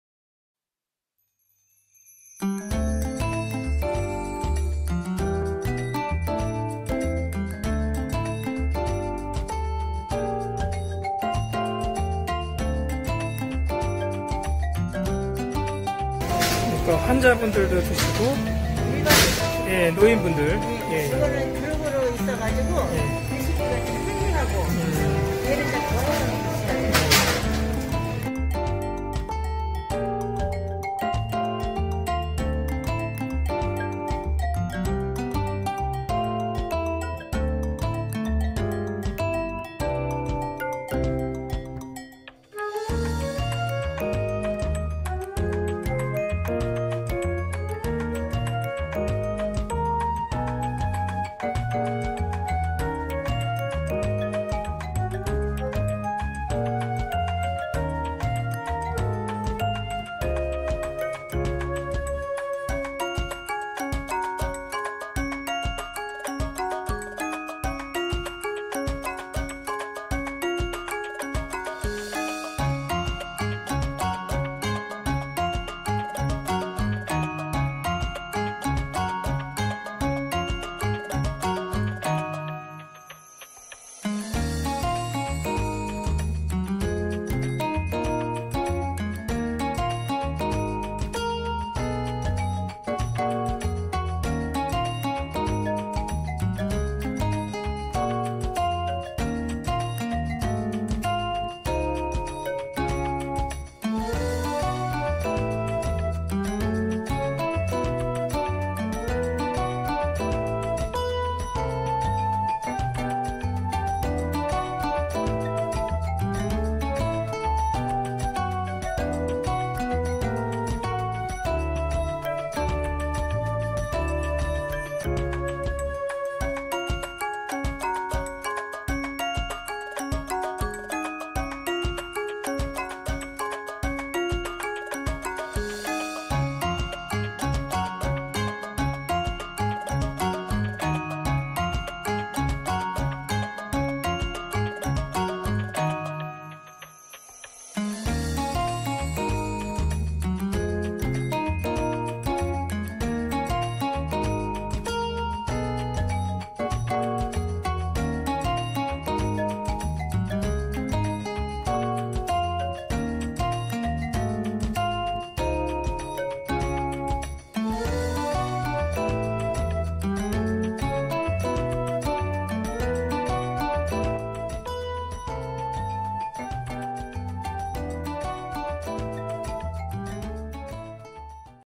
[The BRIDGE_정민두 기자] 초고령사회, 시니어 건강을 챙겨주는 돌봄식 케어푸드!
TIPA 국민참여기자단 The BRIDGE 정민두님의 영상입니다.

돌봄식 케어 푸드는 노인이나 환자 등
특별한 영양공급이 필요한 이들에게 각광받고 있는데요!

특히 고령사회로 진입하면서
케어 푸드에 대한 관심이 높아지고 있습니다.

시니어 건강을 챙겨주는 돌봄식 케어푸드!
앞으로 행보가 기대가 됩니다!

 [중소기업기술정보진흥원 SNS & 페이지]